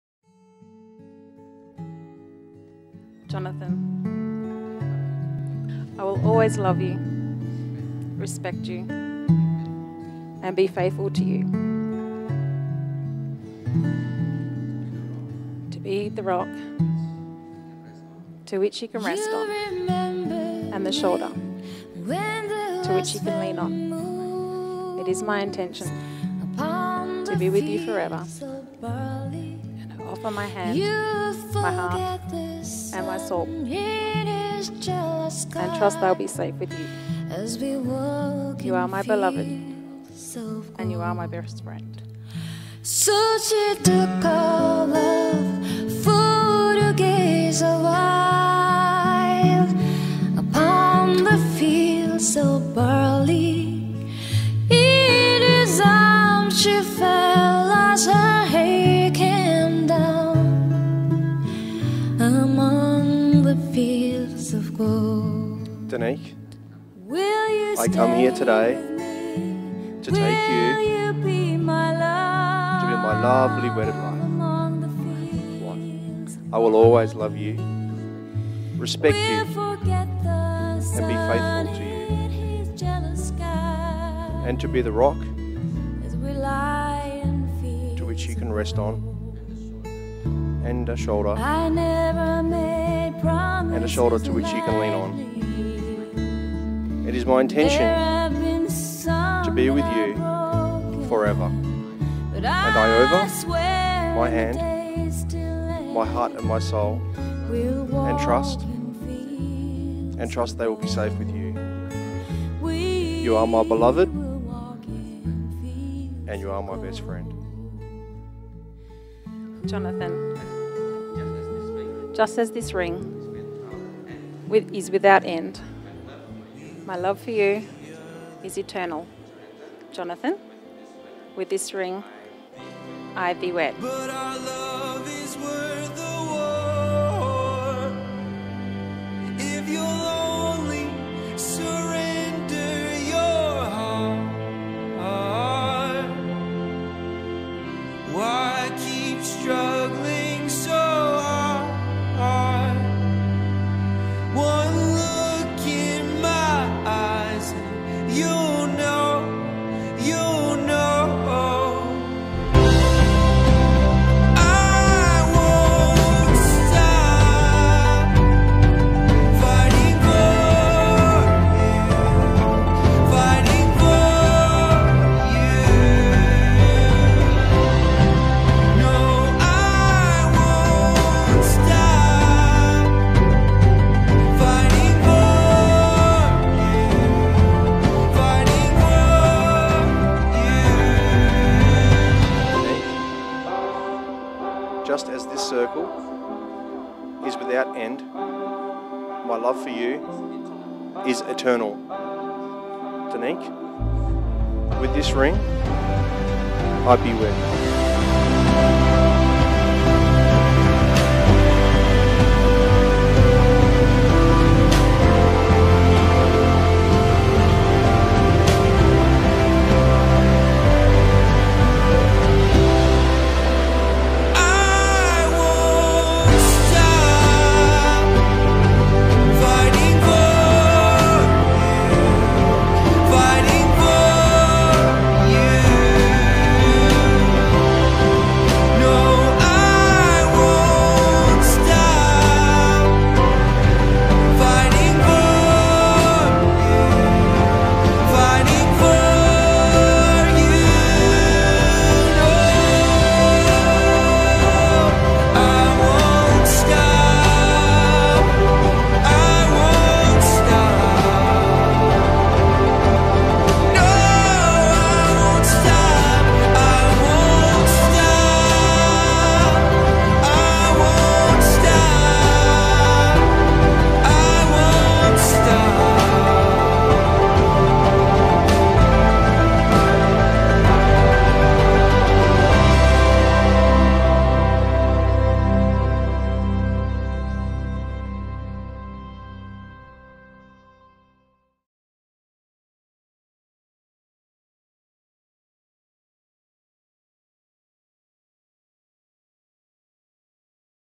Danique & Jonathan - The Wedding 29/12/2016 The Mulia Hotel, Bali
Bali Wedding, planning done by Bali Brides Wedding Planner

Congratulations to Danique & Jonathan on your special day.